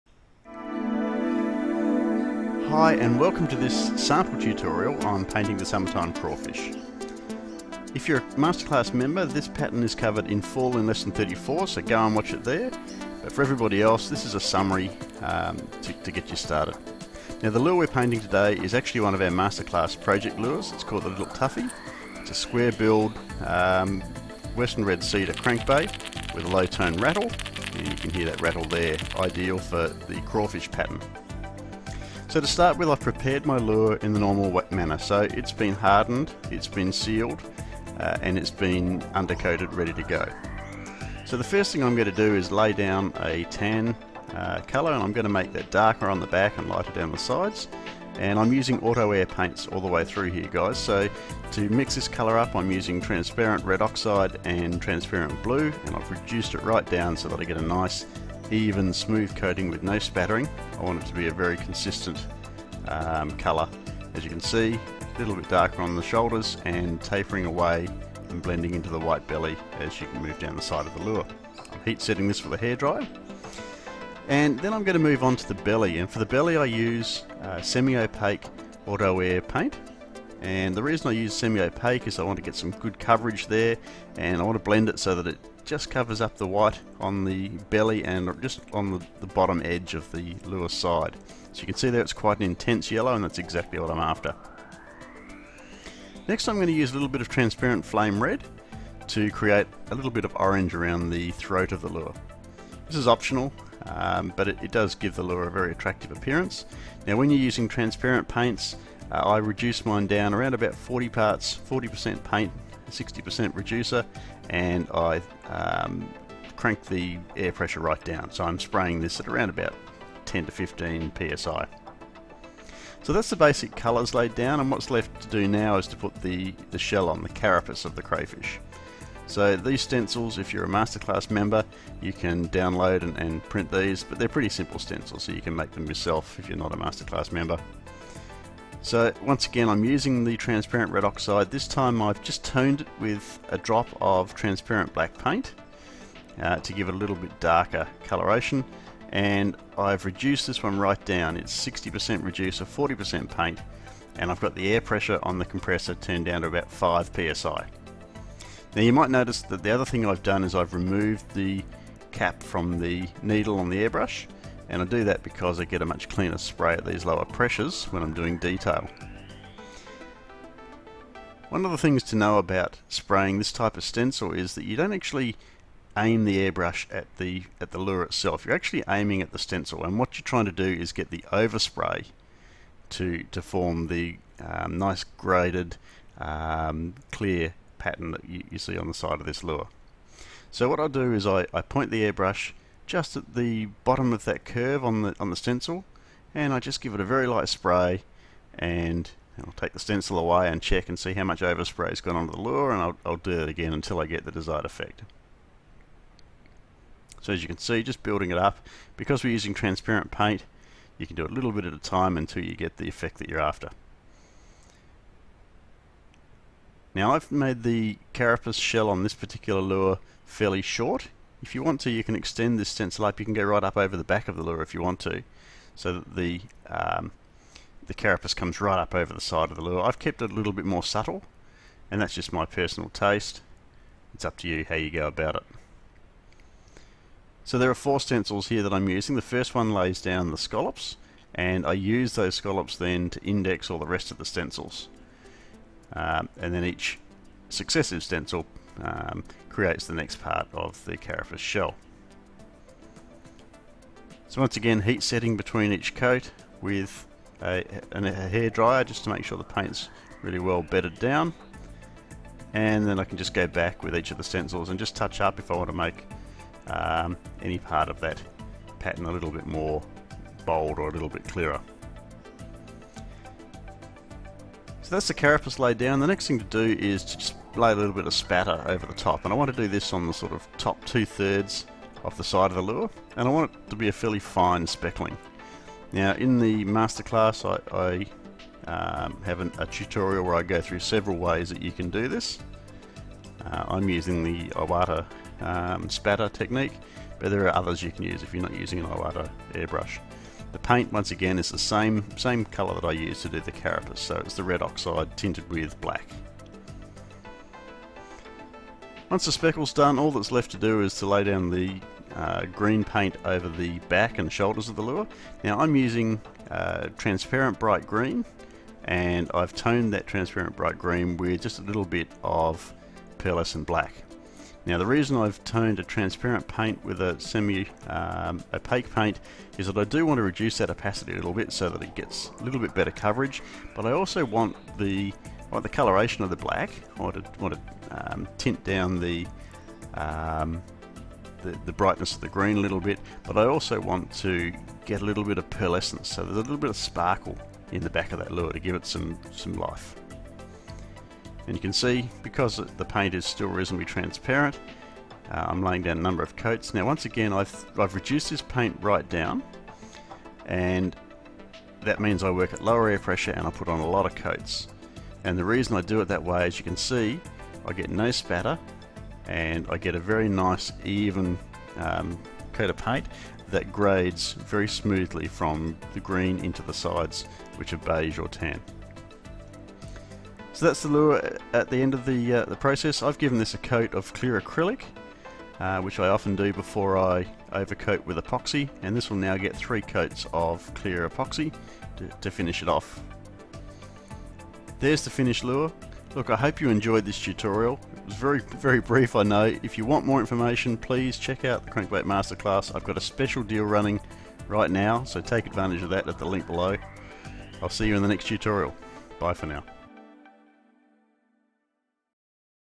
painting summer crawfish pattern
Sample video tutorial on airbrushing a square billed crankbait in summer crawfish colors.